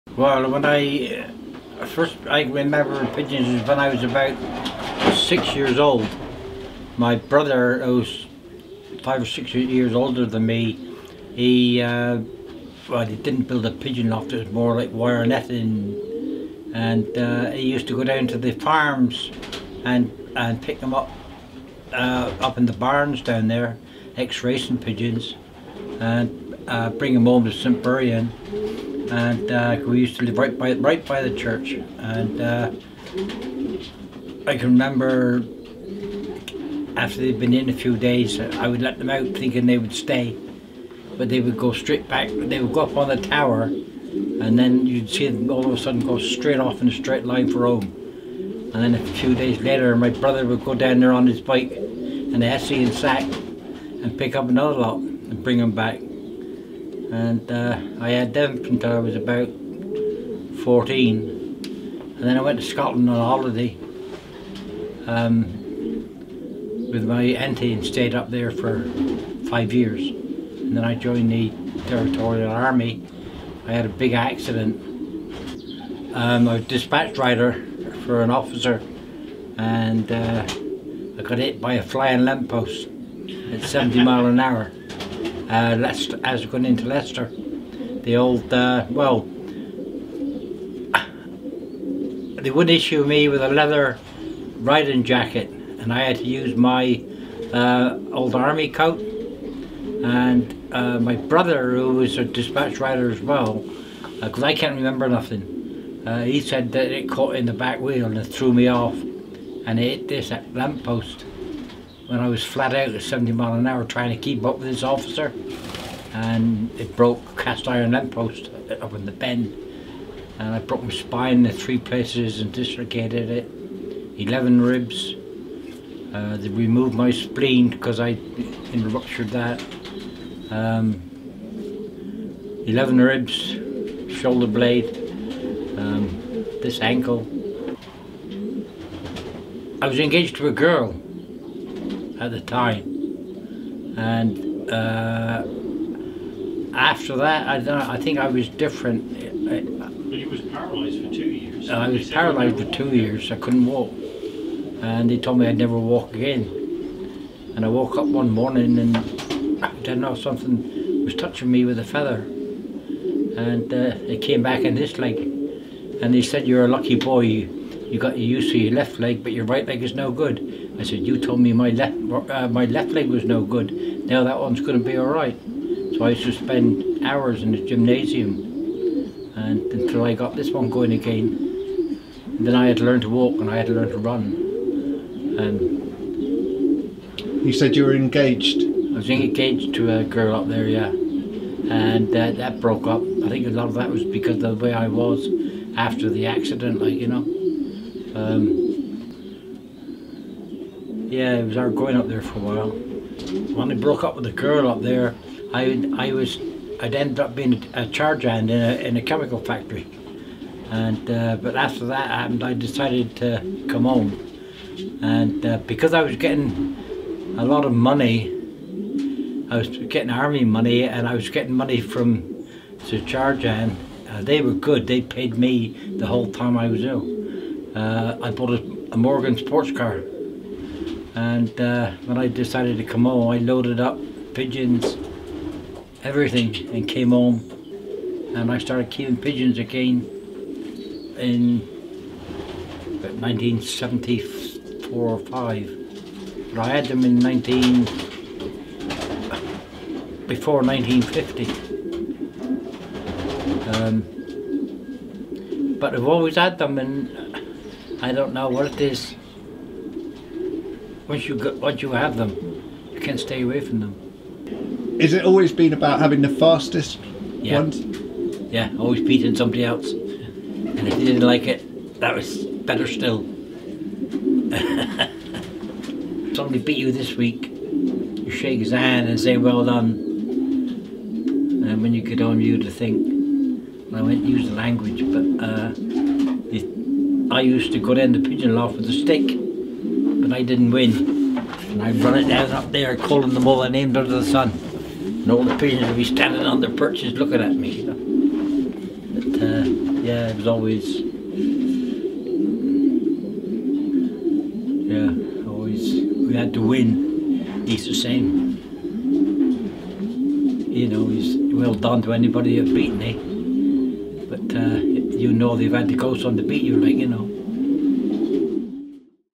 Angwin Family Pigeons How it all began...
Micks' story of a lifelong passion of pigeon racing.
Beautifully captured in a feature by Greg Martin Photojournalist at Cornwall Live.